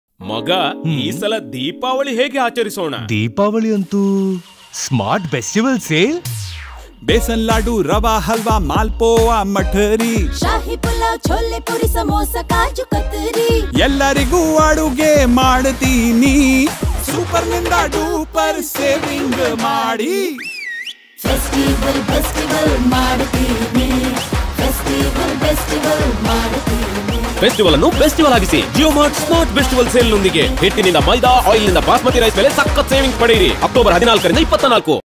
SMART Bestival Sale 14th - 24th Oct | Grocery Generic | Kannada
India’s Biggest Diwali Sale is here! SMARTBestivalSale 14th - 24th Oct. Biggest offers and savings on Dry fruits, Sweets, Chocolates, Oil, Ghee, Dinner sets, Bedsheet, Apparel and more. Visit your nearest SMART Store to avail offers.